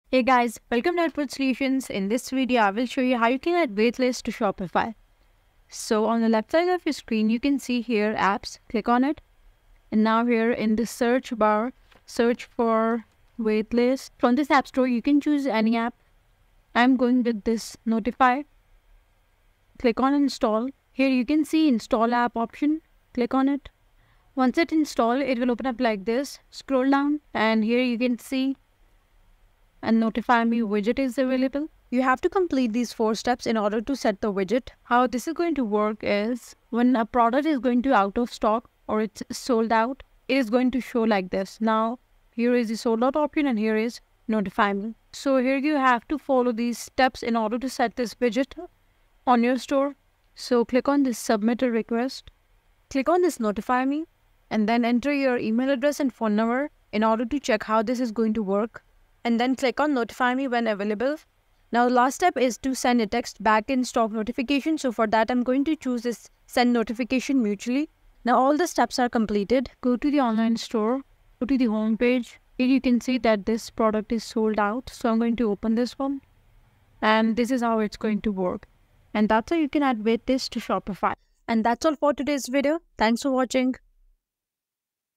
How To Add Waitlist To Shopify (How To Set Up Or Create Waiting List On Shopify)
How To Add Waitlist To Shopify (How To Set Up Or Create Waiting List On Shopify). In this video tutorial I will show how to add waitlist to Shopify.